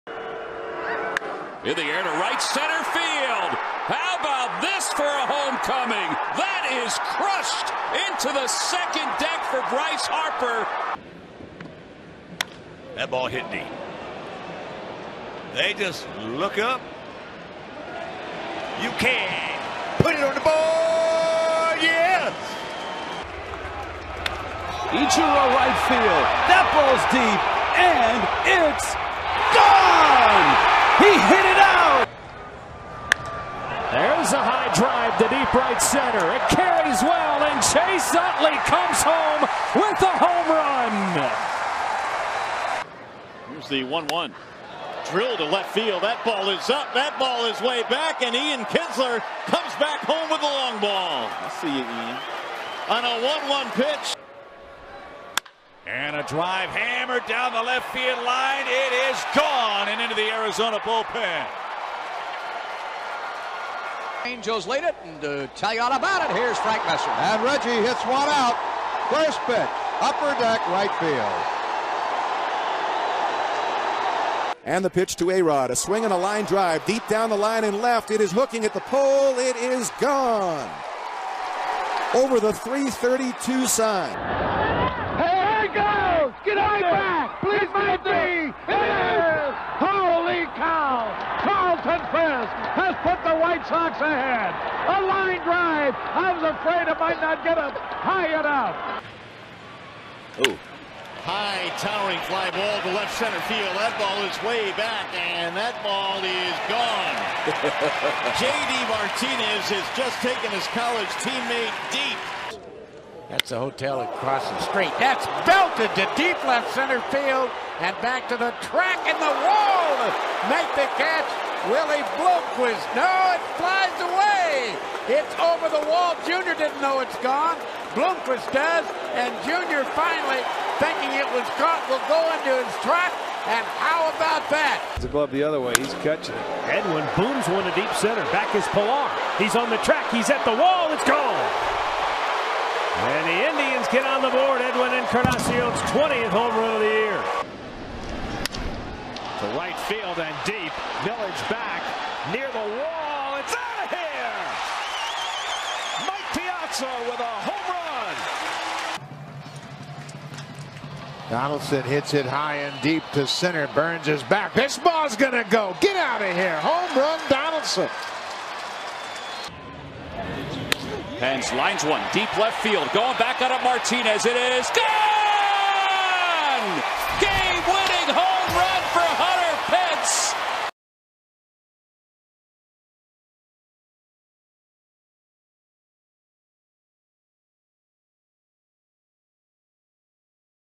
MLB Home Runs vs Former Teams
Players hitting home runs against teams they used to play for